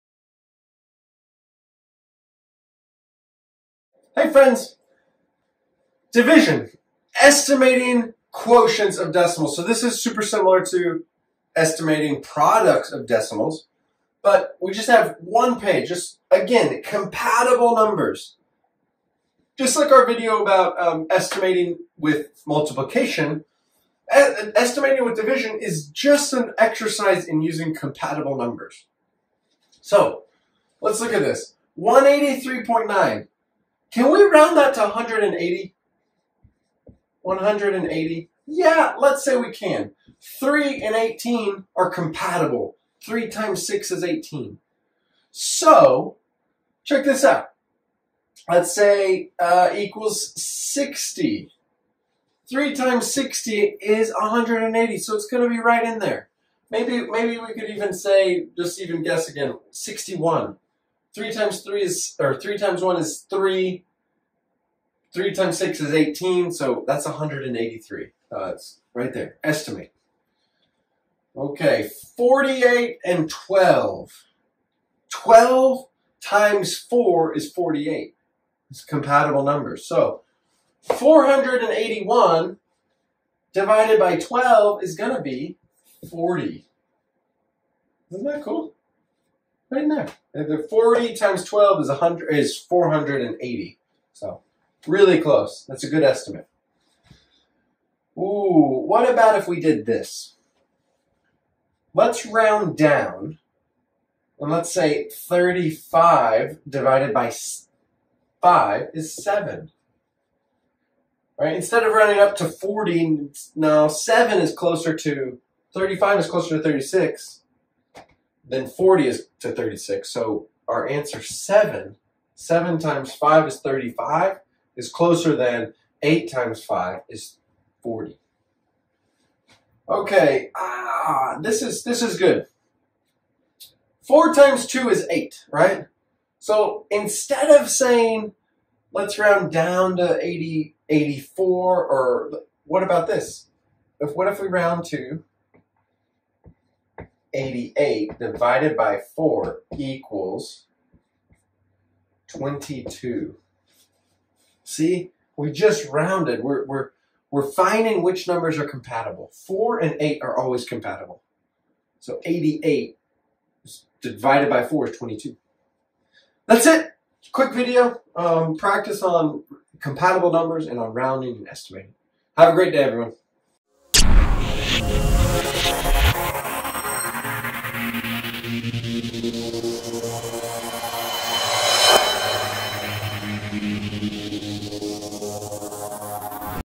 Division - Estimate Quotients of Decimals
Estimate the quotients when dividing decimals.